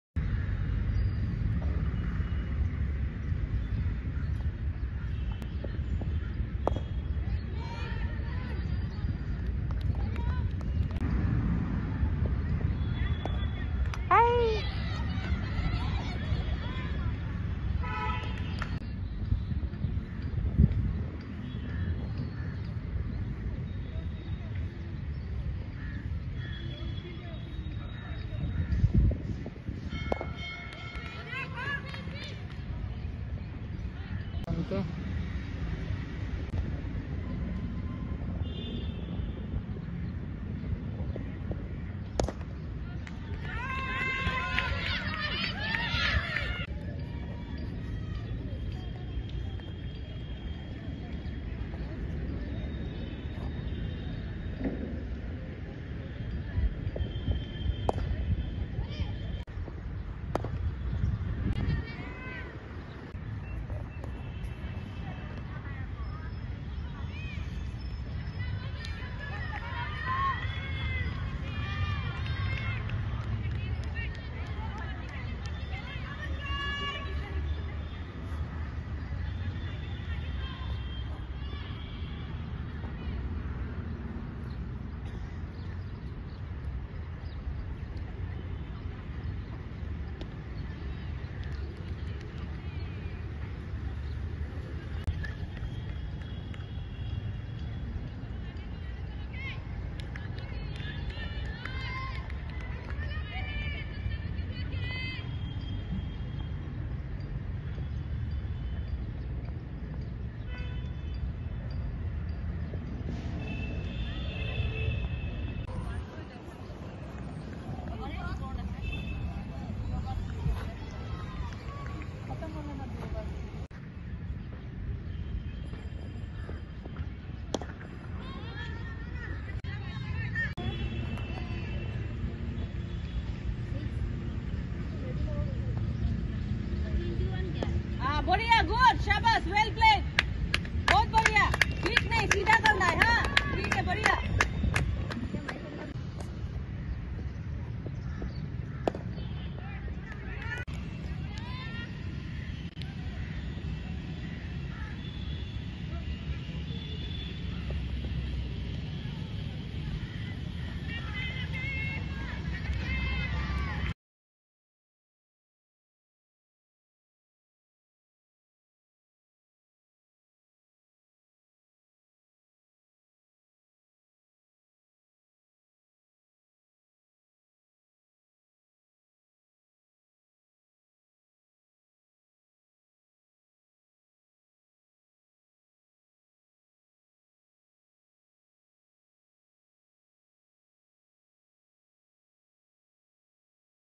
Bukakhat win
কনকলতা বড়ুয়া আন্তঃজেলা সিনিয়র মহিলা ক্রিকেটে বোকাখাতের  কাছে হেরে গেলো হাইলাকান্দি। বর্সাপারা ক্রিকেট স্টেডিয়ামে মঙ্গলবার ম্যাচটি অনুষ্ঠিত হয়।